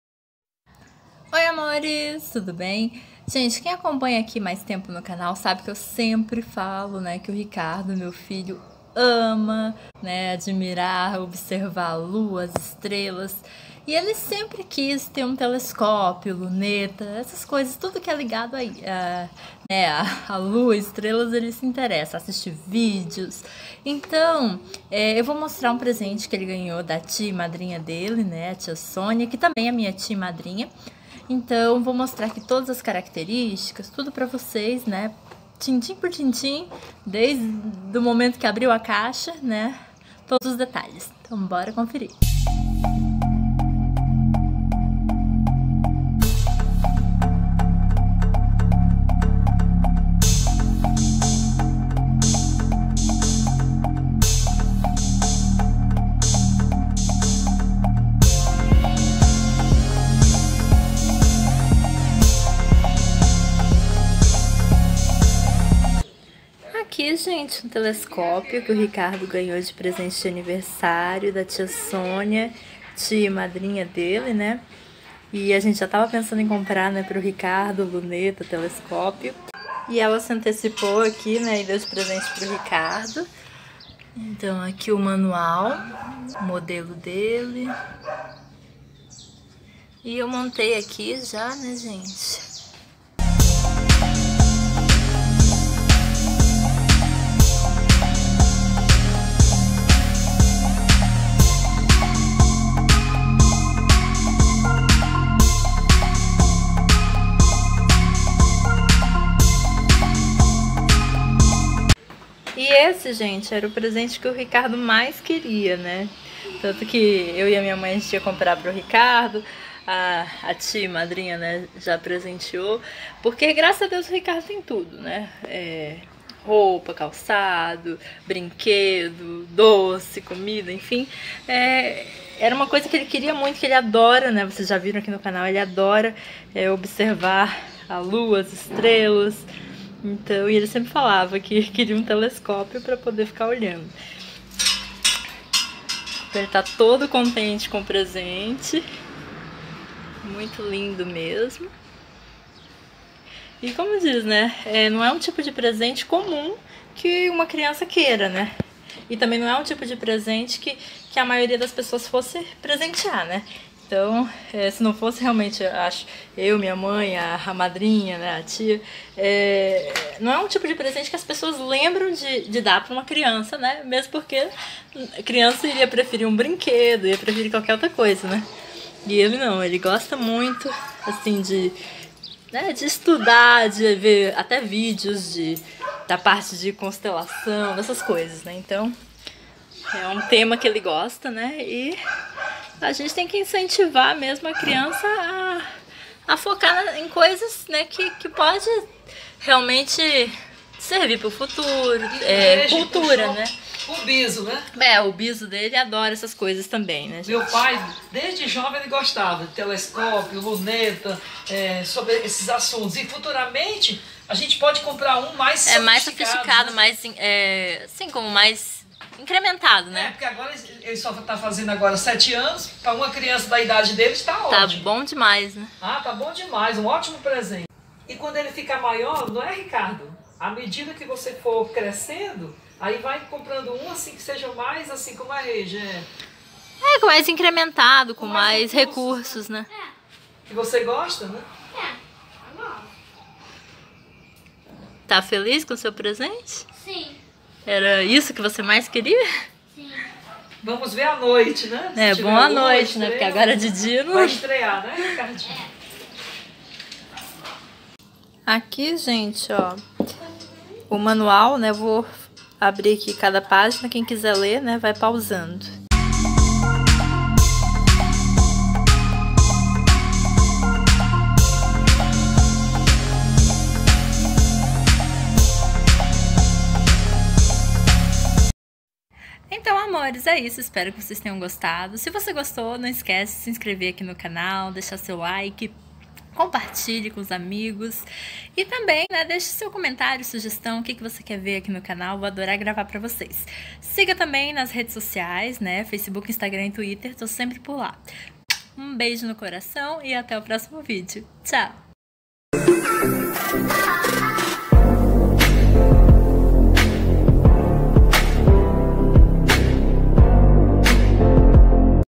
Telescópio Luneta Refrator Constellation F36050 Equifoto Aproximação Até 90x
TELESCOPE OPTICAL GLASS & METAL TUBE F36050
Telescopio Luneta Refrator Constellation F36050 Aproximação Até 90x Observação Lunar Terrestre - A porta de entrada para o universo da Astronomia, ótimo para observar a lua e as estrelas.
- Diâmetro da lente: 50 mm
- Longitude focal: 360mm
- Aproximação: 60 à 90x
- Diâmetro Da Lente: 0,965
- Lentes Oculares: F6 e H20
- Lente Eretora: 1.5x (0,965)
- Espelho Diagonal: Star diagonal 0,965
- Tubo de foco: 0,965
- Tripé em alumínio de 32cm
- Diagonal Híbrida 90°
- Amplitude máxima: 90x
Equifoto